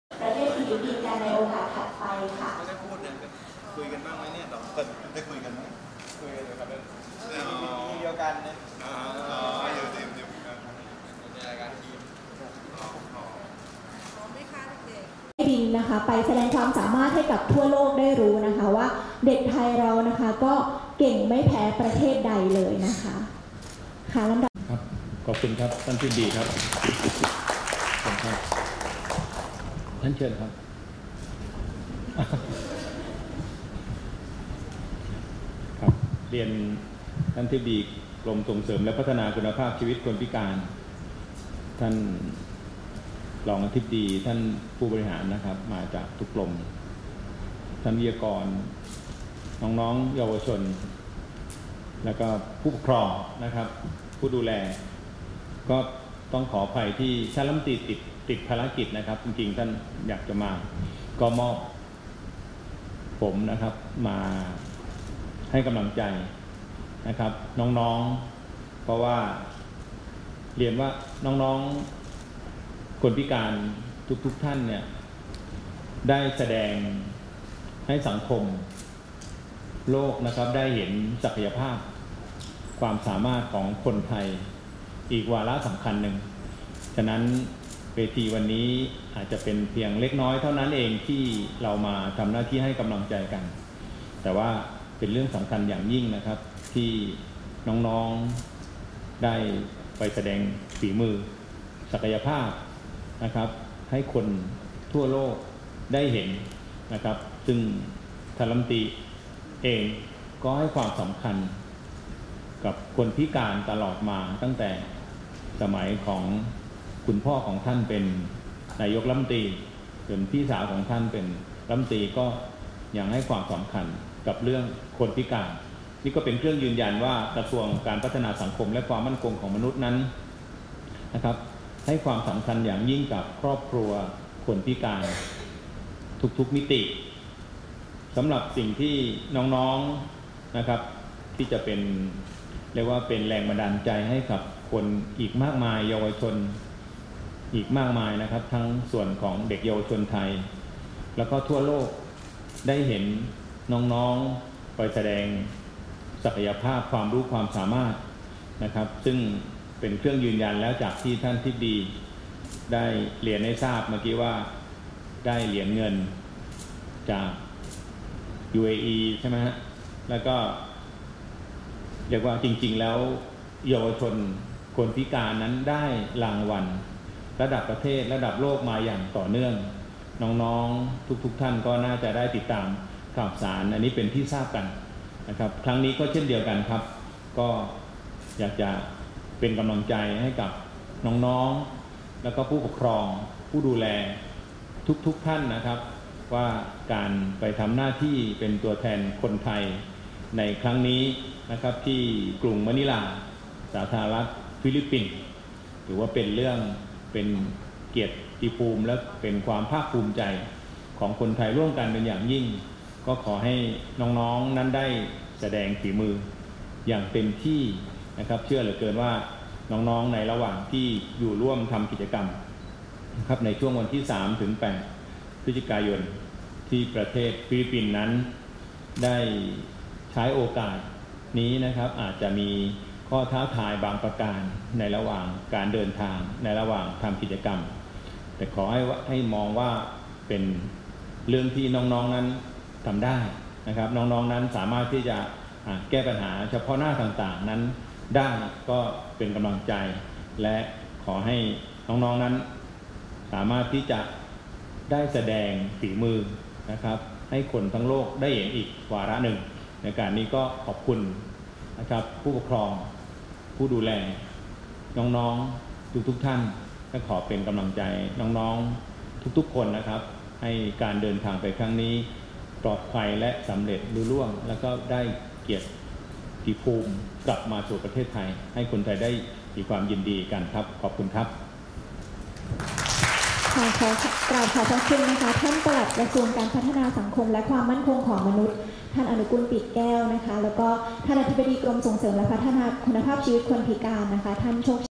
ปลัด พม.ให้โอวาทแก่คณะเยาวชนพิการไทยในการเข้าร่วมการแข่งขันความท้าทายทางเทคโนโลยีสารสนเทศระดับสากล
กระทรวงการพัฒนาสังคมและความมั่นคงของมนุษย์

วันที่ 31 ตุลาคม 2567 เวลา 13.30 น. นายอนุกูล ปีดแก้ว ปลัดกระทรวงการพัฒนาสังคมและความมั่นคงของมนุษย์ (ปลัด พม.) กล่าวให้โอวาทแก่คณะเยาวชนพิการไทยในการเข้าร่วมการแข่งขันความท้าทายทางเทคโนโลยีสารสนเทศระดับสากล ประจำปี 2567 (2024 Global IT Challenge for Youth with Disabilities) โดยมี นายโชคชัย วิเชียรชัยยะ อธิบดีกรมส่งเสริมและพัฒนาคุณภาพชีวิตคนพิการ กล่าวรายงาน พร้อมด้วยผู้บริหารกระทรวง พม. เข้าร่วม ณ โรงแรมรอยัล ริเวอร์ กรุงเทพมหานคร